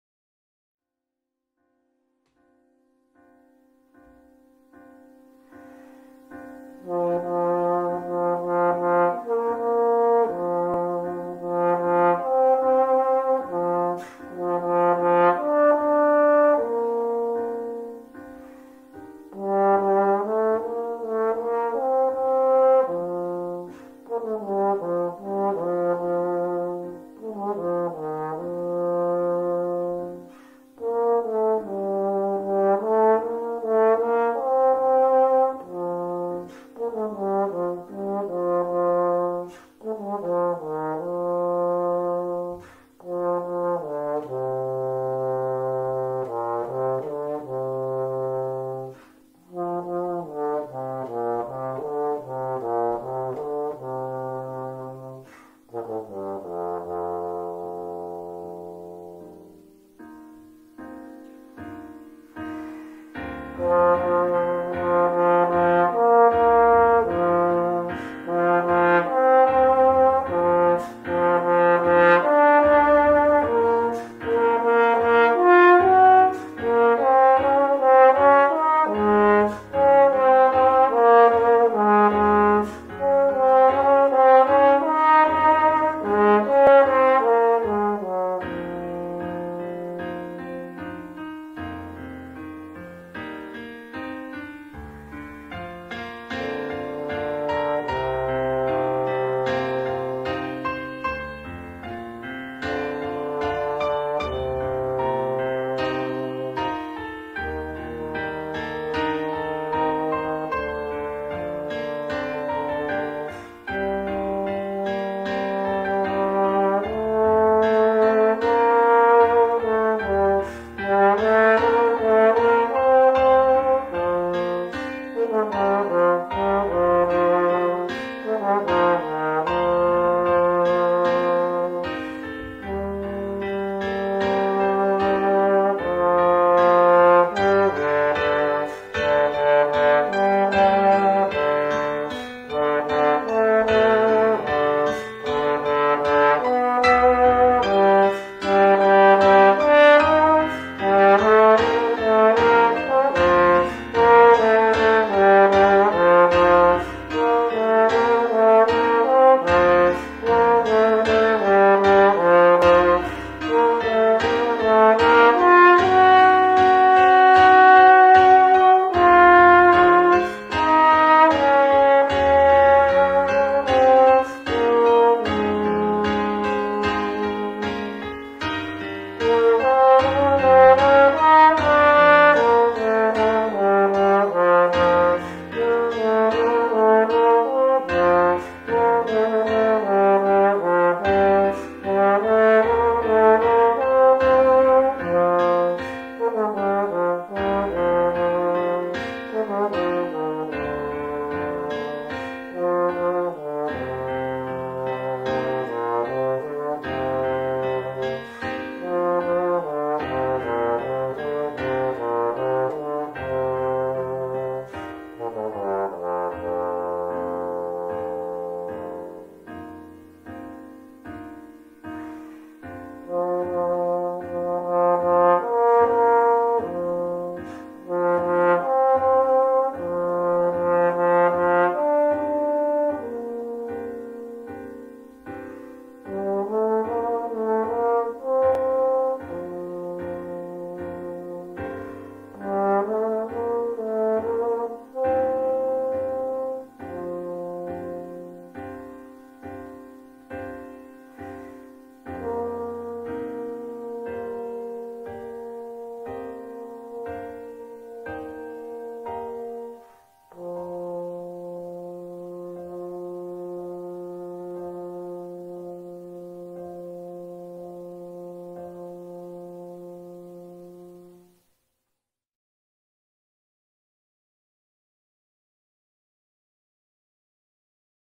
Best of the Living Room Series vol. 6
John Sebastian Vera plays live from his living room.